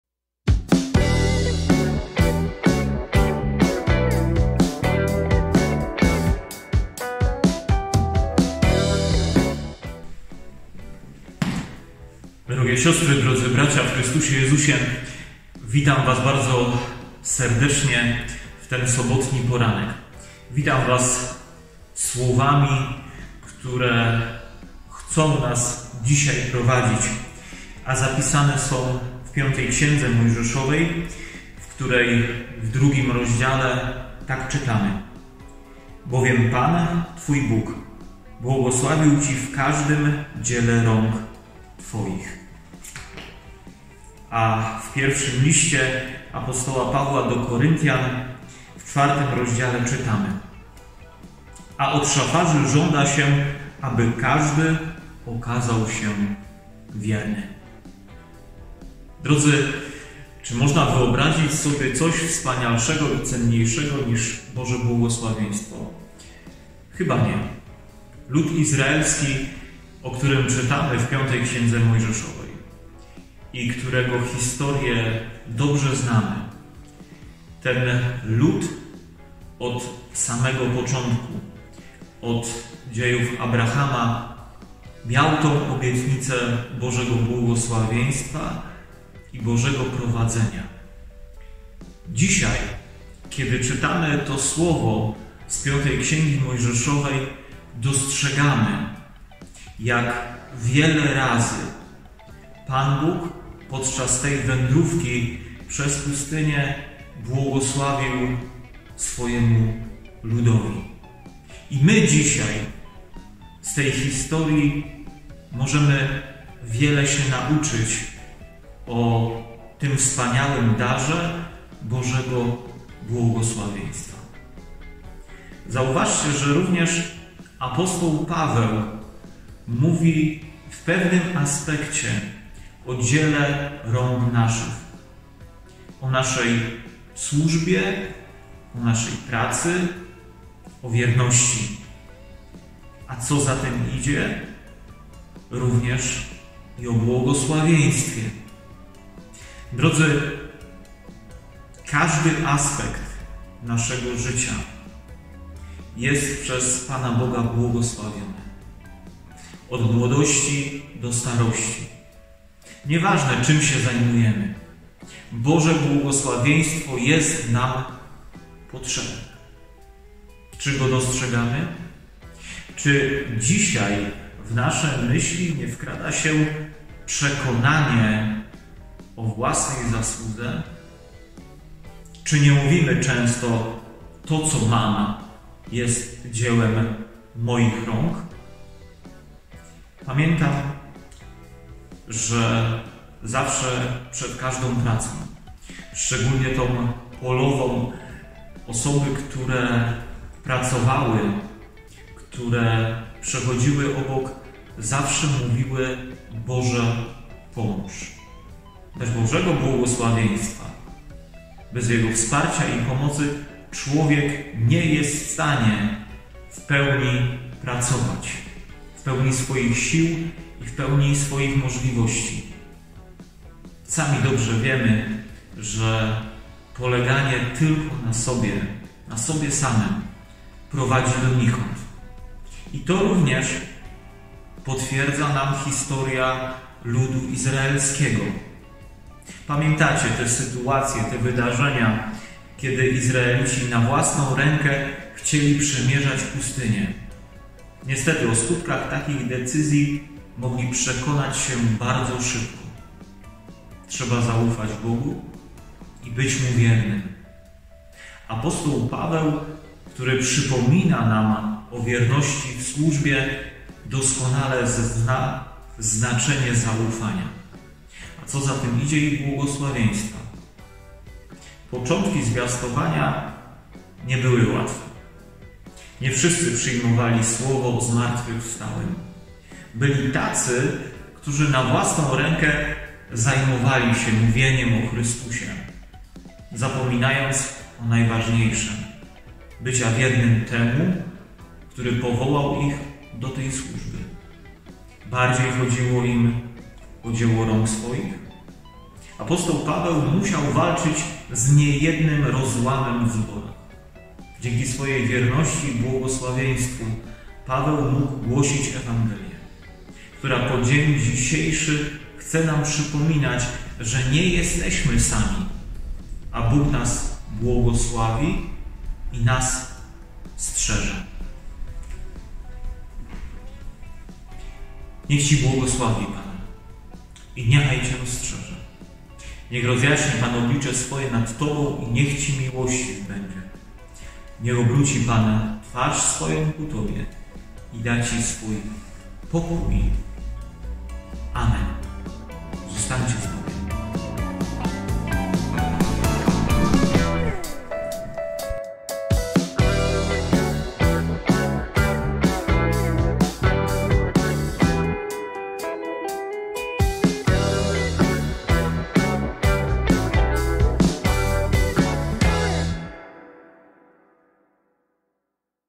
Codziennik biblijny, Słowo na dzień 9 maja 2020 r.
ks. Dariusz Chwastek, proboszcz parafii ewangelicko-augsburskiej w Nowym Sączu.